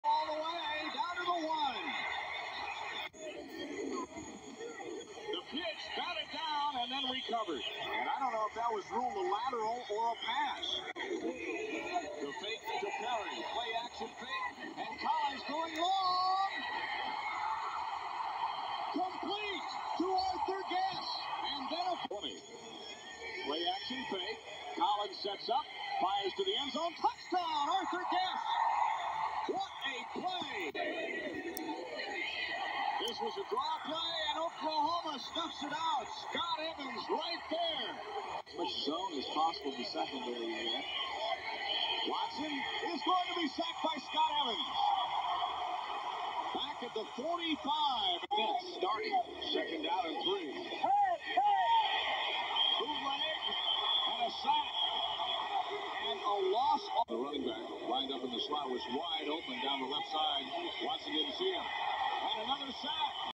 OU vs Kansas state 1989 highlights from thegoodmangino
This video is Oklahoma 42 Kansas state 19 and this was on sooner television and Also at night/evening and lastly Oklahoma won a game that has fun. Earlier in the season we destroyed New Mexico State by a score of 72-3 good offense and also a dominating defensive thame.